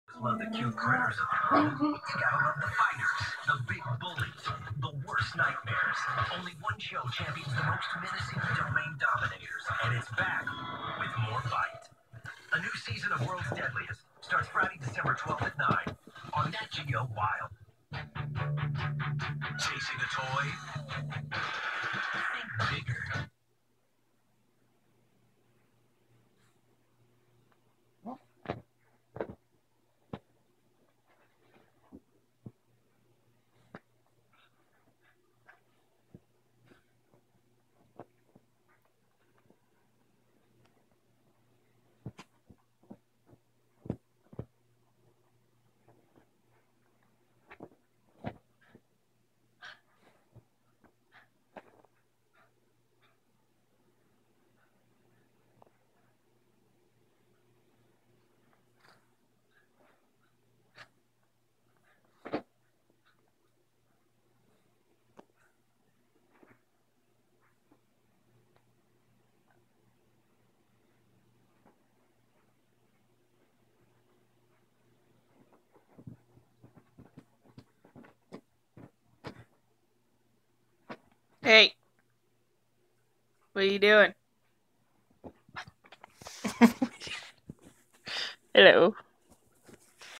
Cockapoo Athena's normal night
Athena destroying the living room per usual.